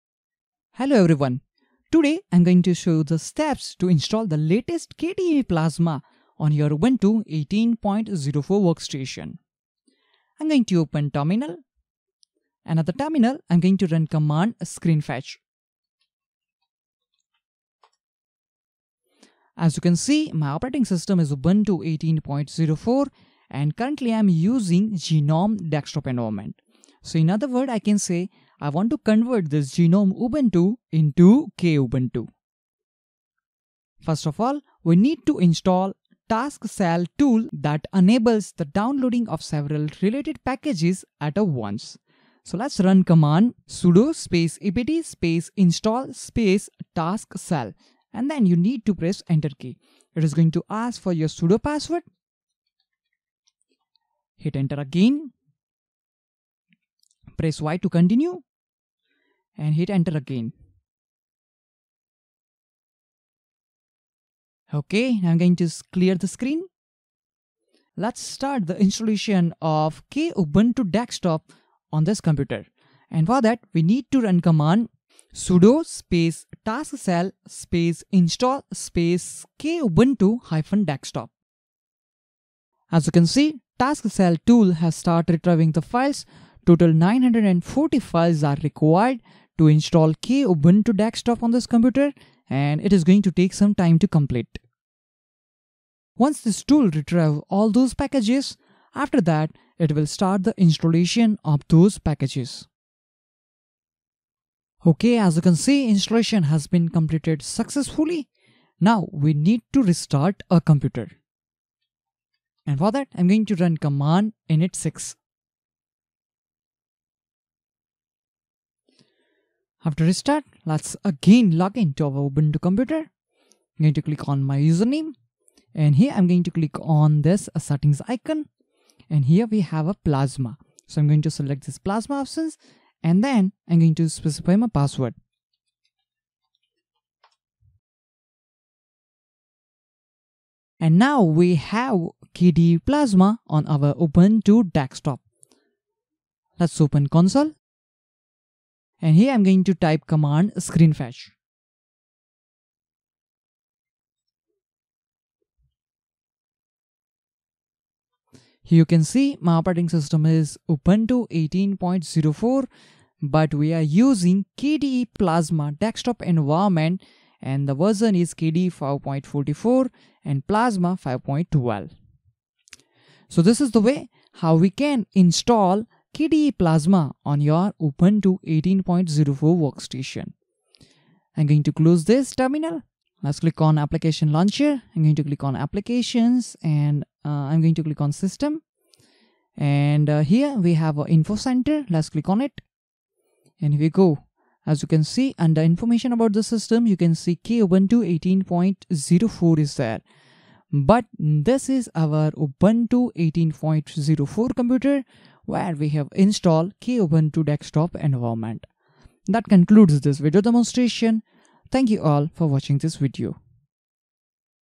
Installing the Full Kubuntu Desktop in Ubuntu 18 04
Ubuntu 18.04 Beginners Video Tutorials:

In this video, I will show you how to install KDE Plasma Desktop and use its sddm display manager for your Ubuntu 18.04 Workstation.

1) Install Tasksel
sudo apt install tasksel

2) Install Kubuntu Desktop
sudo tasksel install kubuntu-desktop

3) Restart your system to login to KDE Plasma.